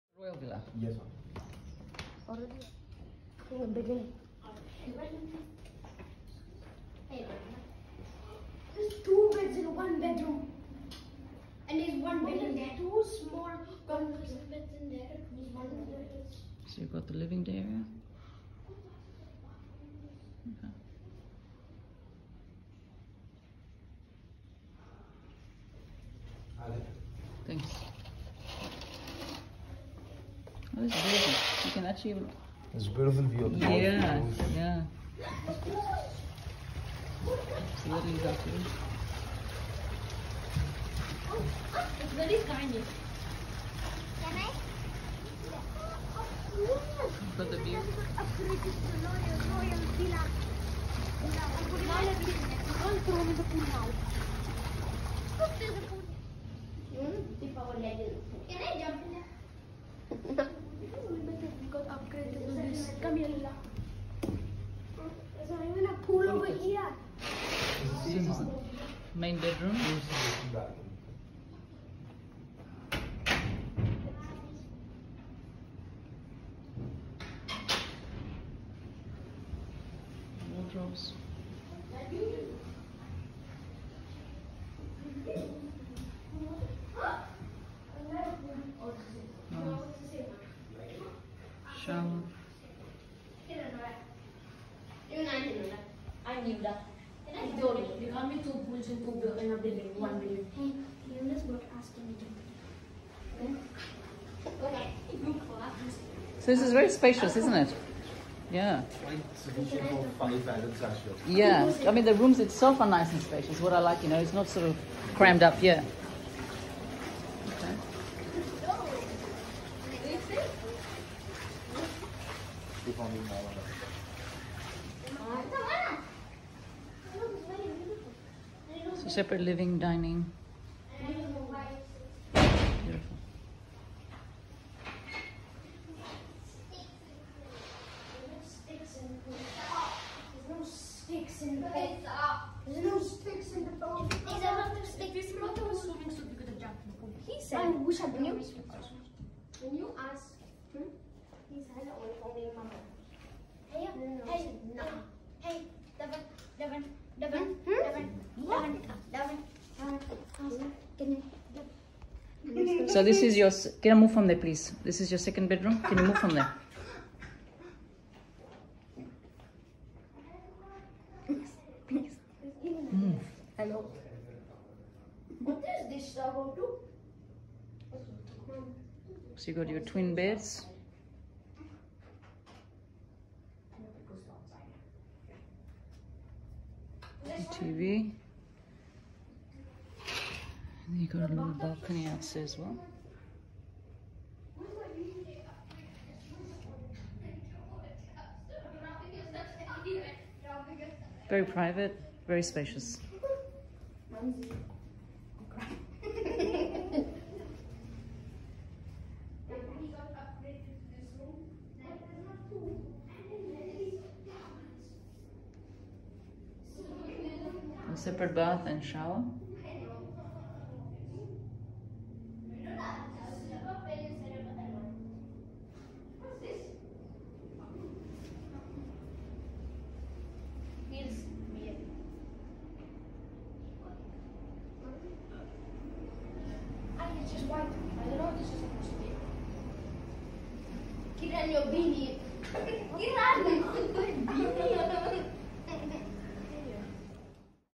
Jump Into Your Own Private Pool At the 5 star The St. Regis Goa Resort, John Jacob Astor Villa
Have a look at the luxurious 5 star The St. Regis Goa Resort Hotel (formerly Leela Goa) in Goa, India. This is the beautiful two bedroom John Jacob Astor Villa.

The St. Regis Goa Resort offers a luxury experience in South Goa, India, set amidst 49 acres of lush, sprawling greenery and sparkling lagoons.

Dine on local cuisine at their restaurant, have afternoon tea at The Drawing Room, indulge in seafood and grilled meats at Susegado or sip cocktails at The Lobby Bar. This is a great place to pamper yourself with Ayurvedic and holistic therapies at the spa, or tee off at the12-hole golf course overlooking the Arabian sea.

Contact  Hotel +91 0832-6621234